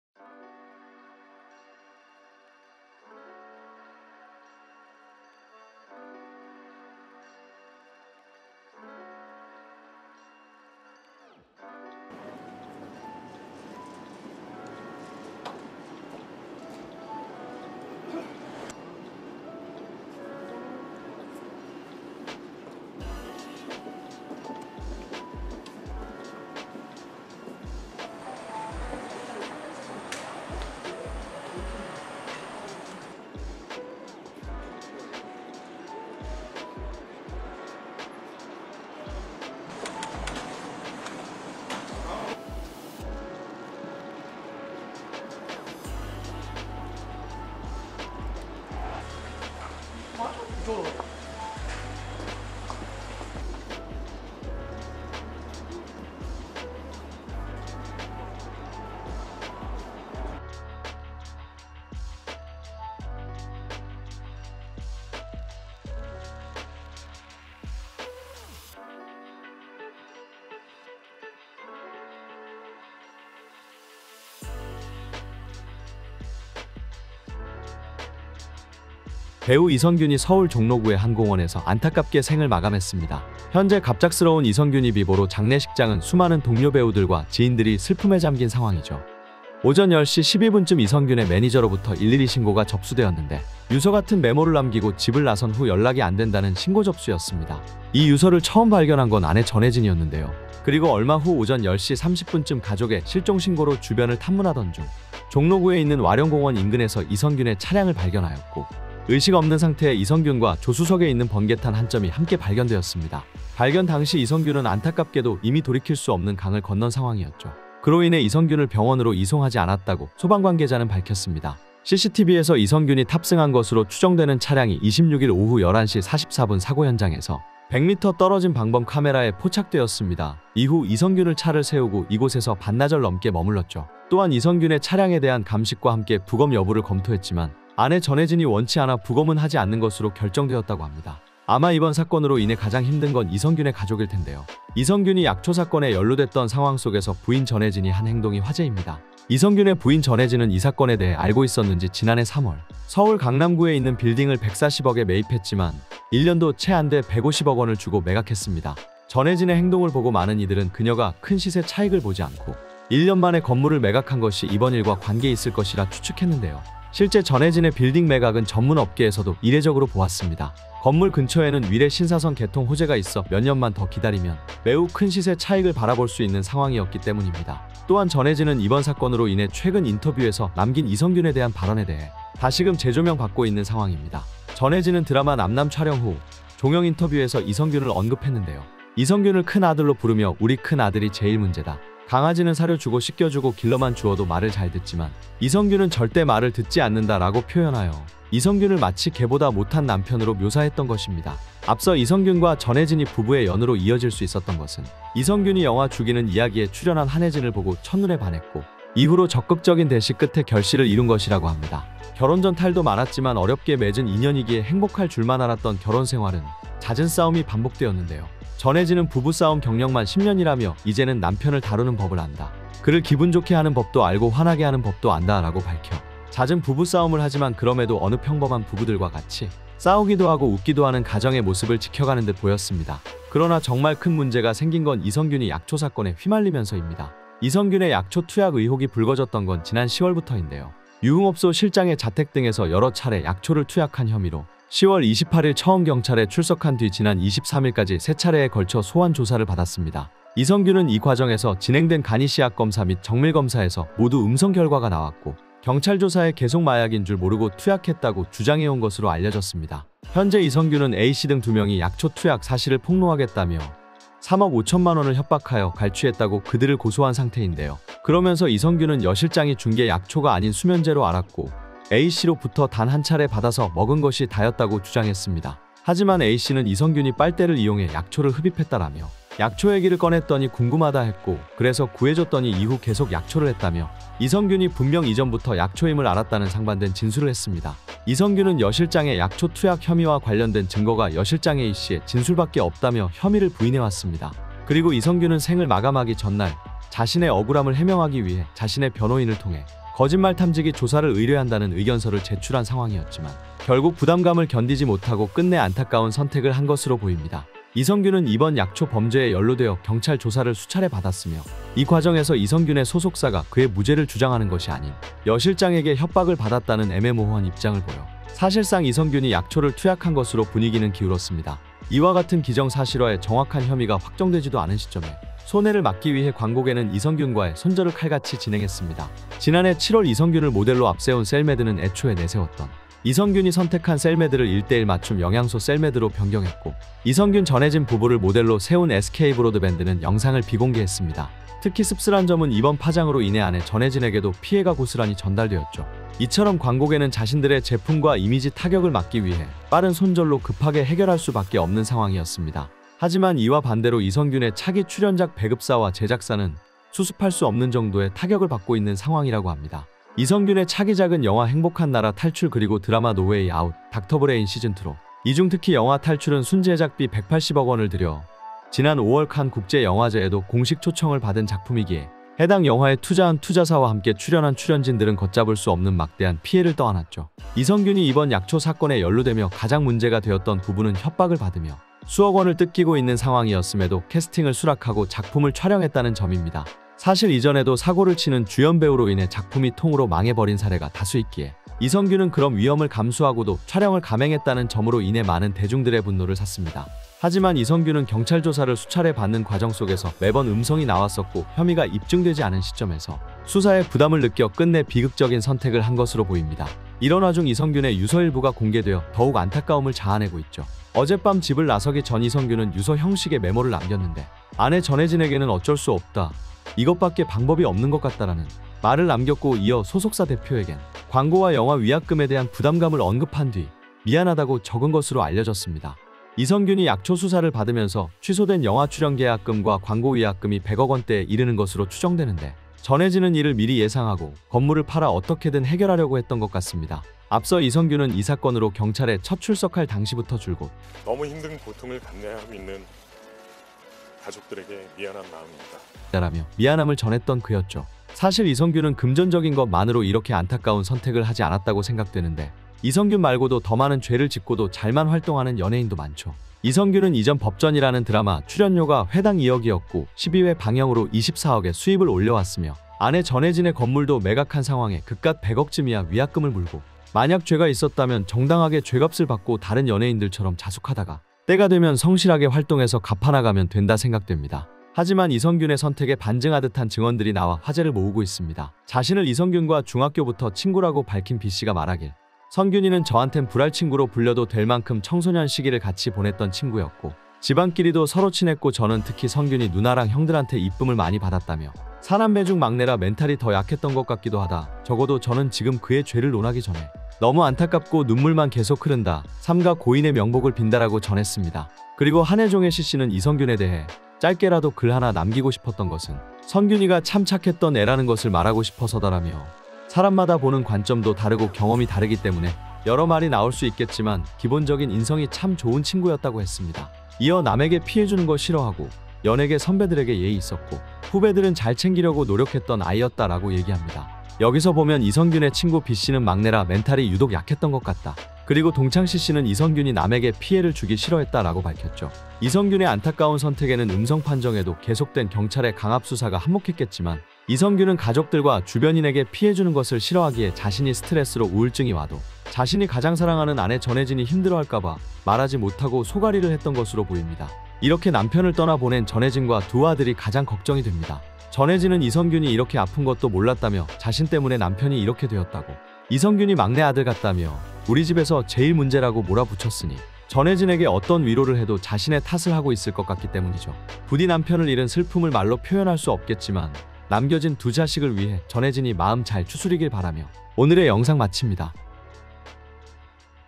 [최초 미공개 빈소 영상] 이선균 빈소에서 끝까지 울지 않던 전혜진이 결국 김마담의 진실을 접하고 통곡하자 흐느낀 조문객들 | "김마담과 당신은 사람이 아니다"
고인이 된 배우 이선균과 전혜진의 근황과 더불어 일화를 전해드립니다.